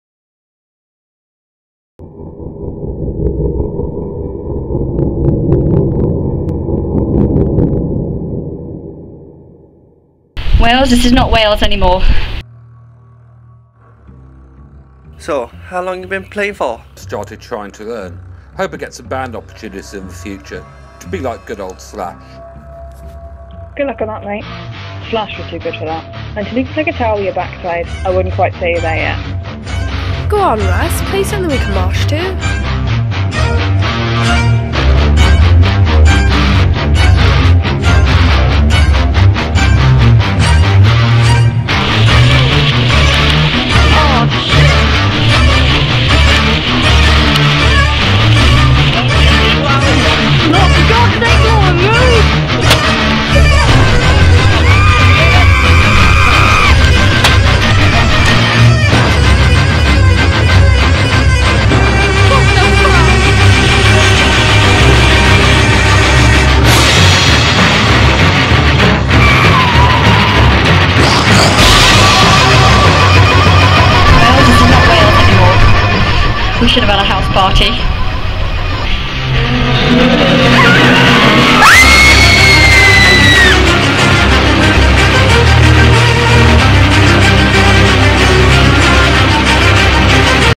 MS3 Media coursework- a zombie film trailer
My MS3 Media coursework, A zombie film trailer ! Me and my friends made this short film/ movie trailer.